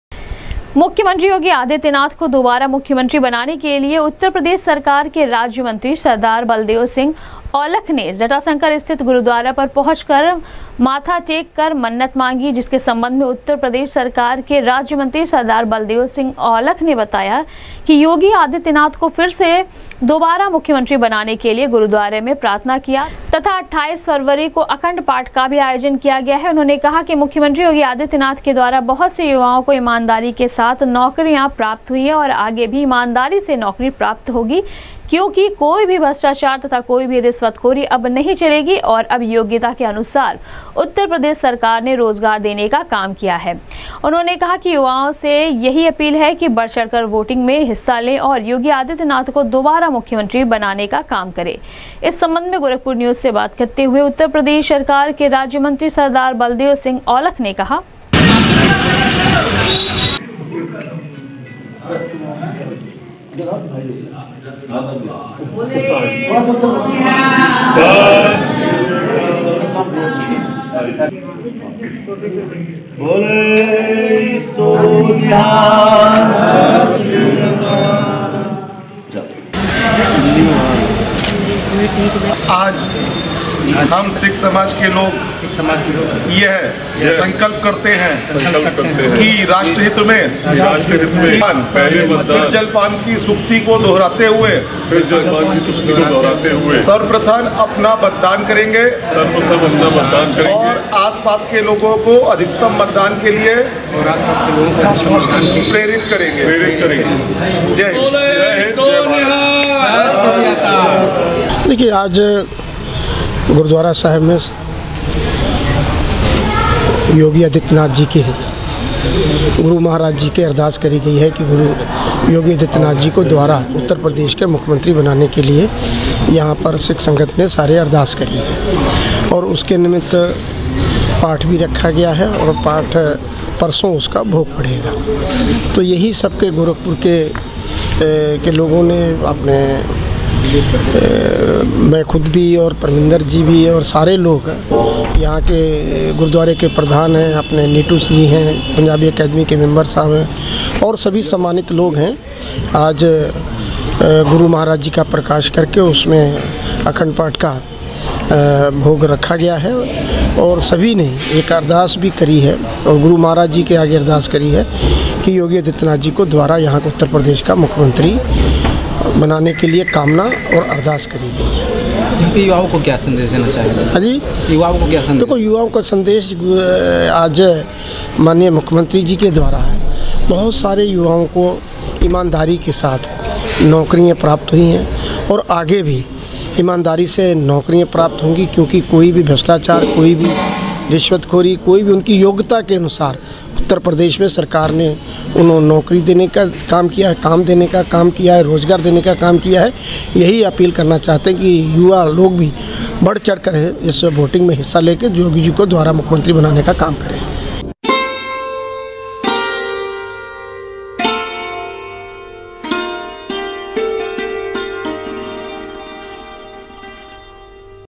प्रदेश सरकार के राज्यमंत्री सरदार बलदेव सिंह औलख ने जटाशंकर स्थित गुरुद्वारा पर पहुंचकर मत्था टेककर
प्रदेश सरकार के राज्यमंत्री सरदार बलदेव सिंह औलख ने जटाशंकर स्थित गुरुद्वारा पर पहुंचकर मत्था टेककर मुख्यमंत्री योगी आदित्यनाथ को दोबारा मुख्यमंत्री बनाने की मांगी मन्नत, बढ़-चढ़कर वोटिंग में हिस्सा लेने की युवाओं से की अपील